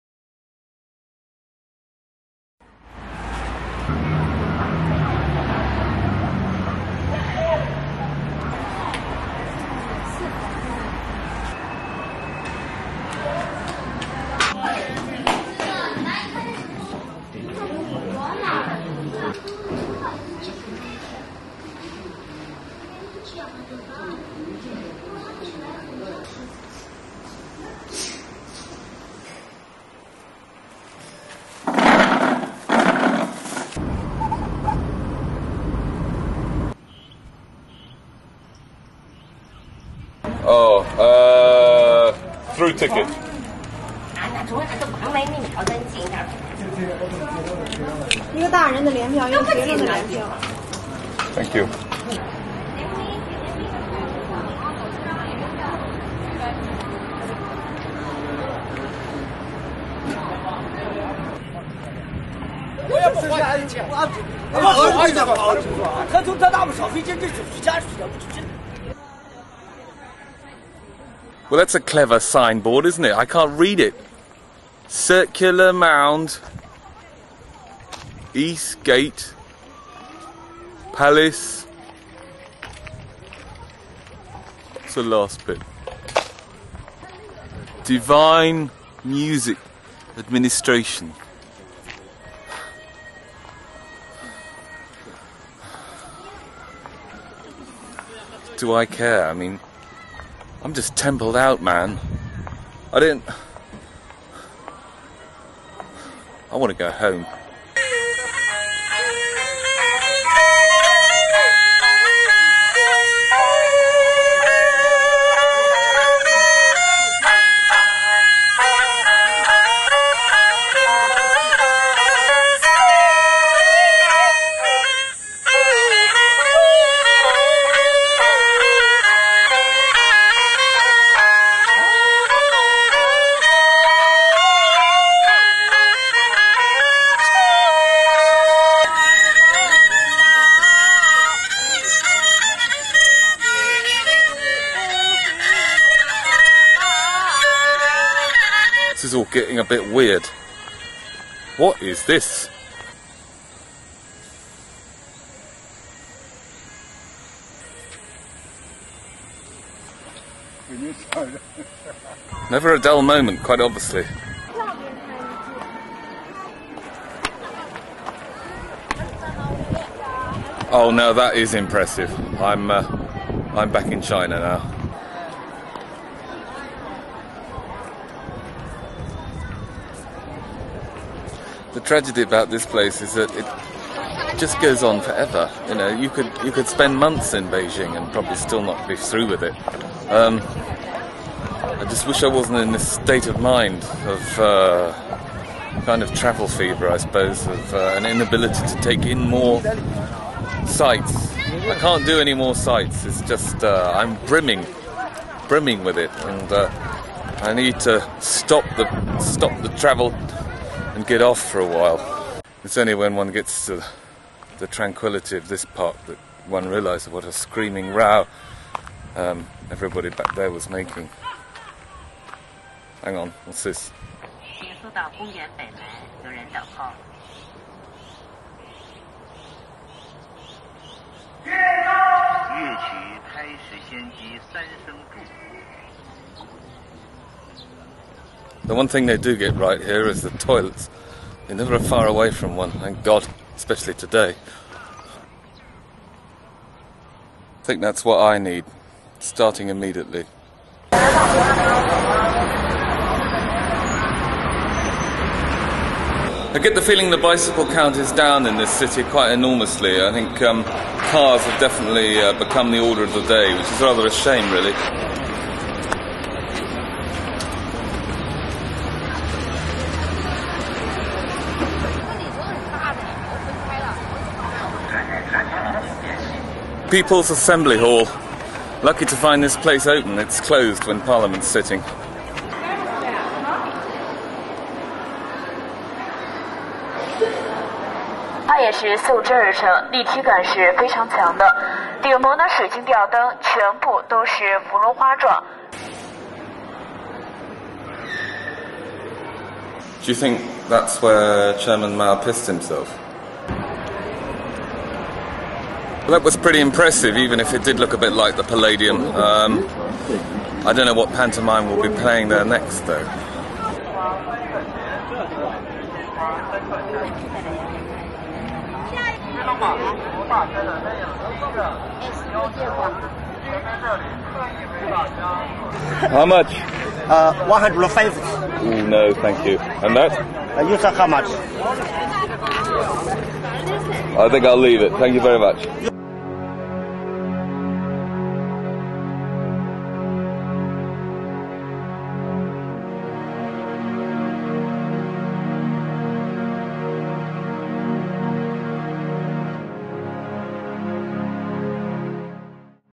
Travels In China, Part 10 : Beijing (contd.)
In which I succumb to temple overload at the Temple of Heaven, but still manage to view a variety of hotspots in the Beijing area.